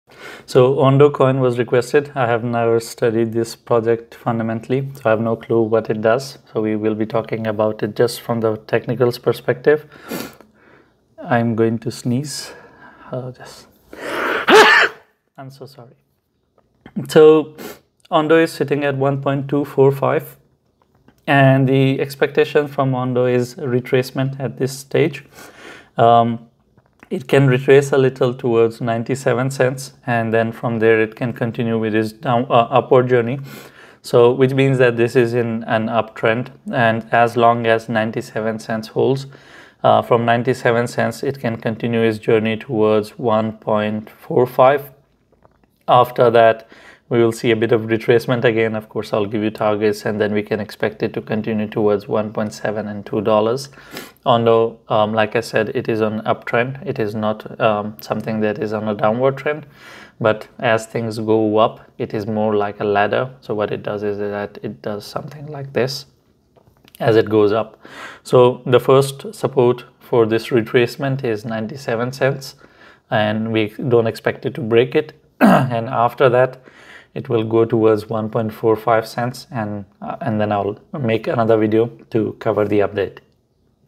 Ondo ONDO Price Prediction 28 May 2024
Welcome to the Shah Faisal Shah Channel!

Welcome to the daily ONDO Ondo price prediction for May 28, 2024. In this video, we'll dissect the current market trends, technical indicators, and recent developments impacting Ondo's price trajectory. Join me as we delve into insightful analysis and forecasts for ONDO, offering potential scenarios for its movement in the crypto landscape.

Your engagement and comments are essential as we navigate the dynamic world of cryptocurrencies.